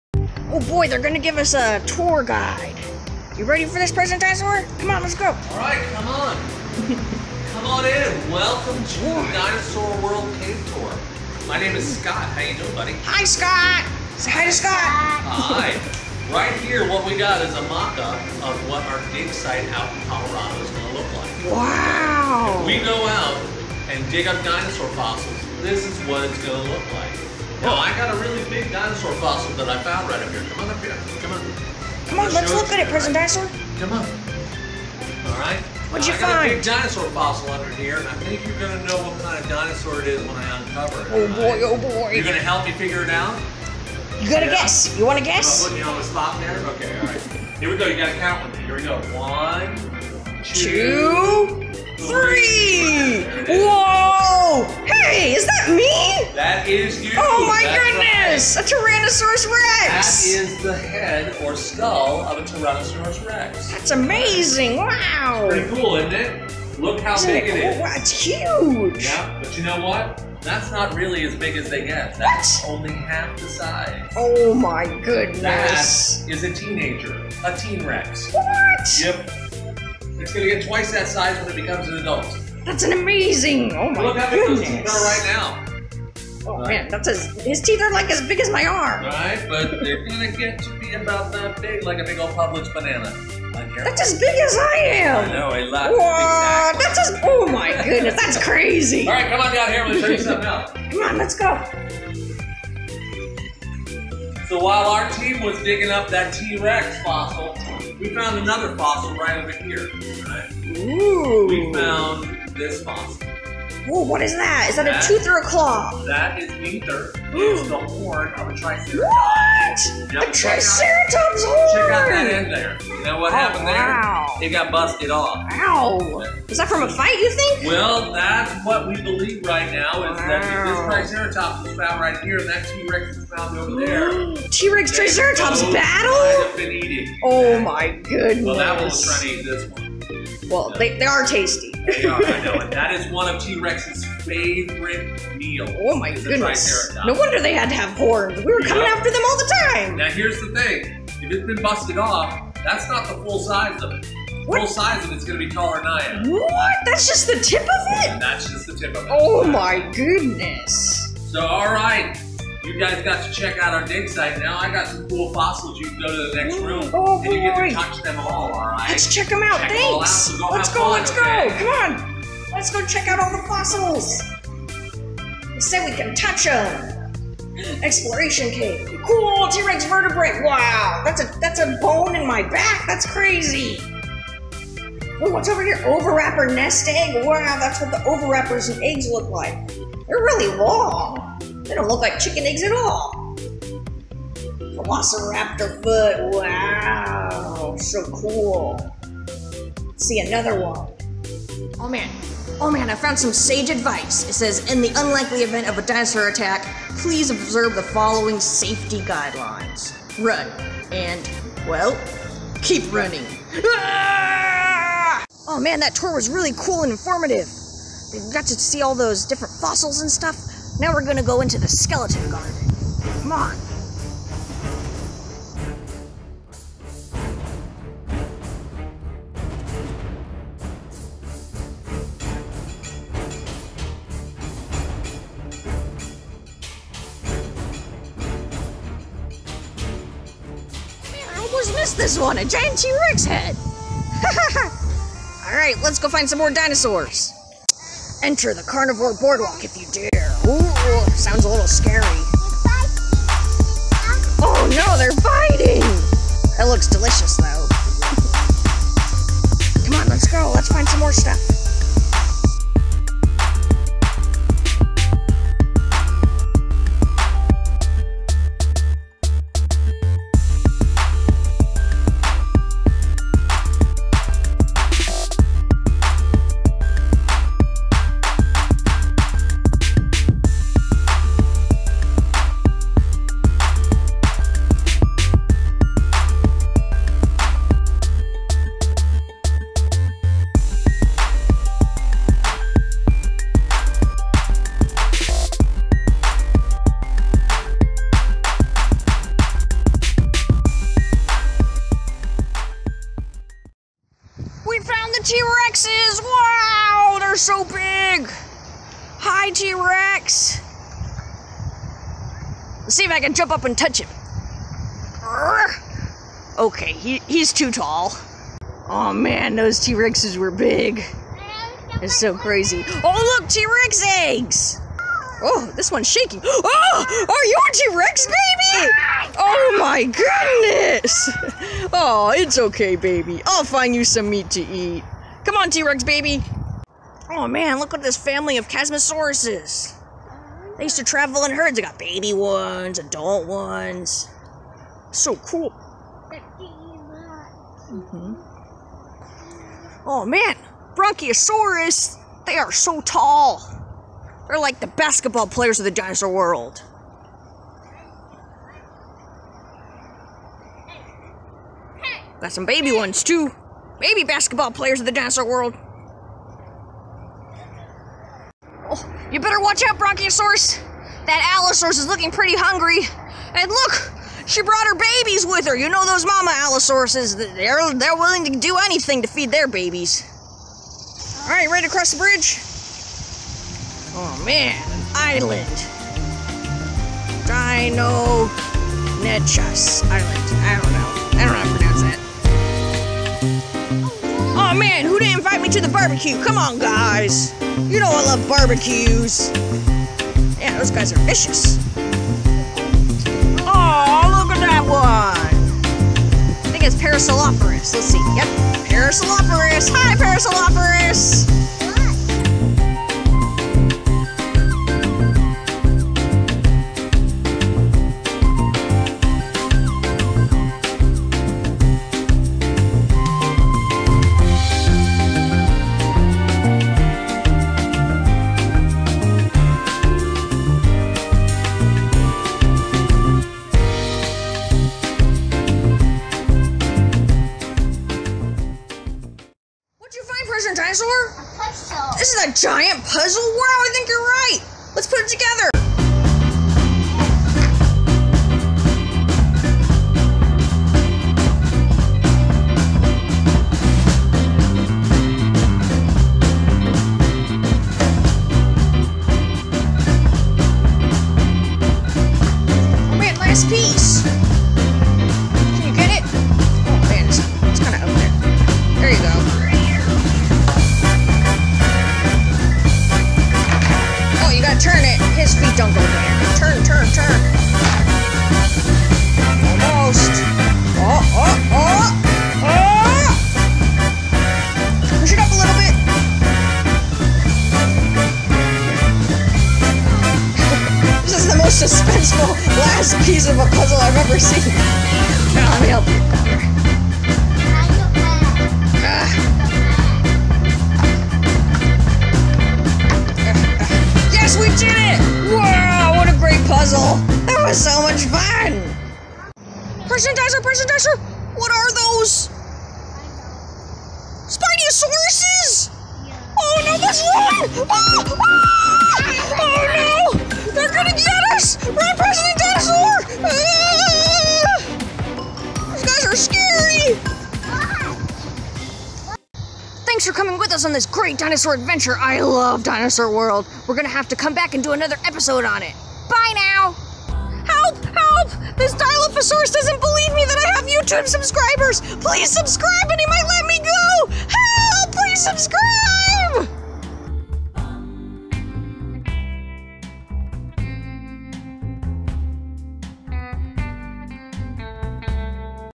Marco the Dinosaur goes to DINOSAUR WORLD! PART2 (D is for Dinosaur) Dinosaur Adventure
Part 2 of Marco's adventure in Dinosaur World! Starts with the archaeologist tour, the carnivore boardwalk, GIANT wooden dinosaur puzzle, Being chases by spinosauruses! Loads of fun.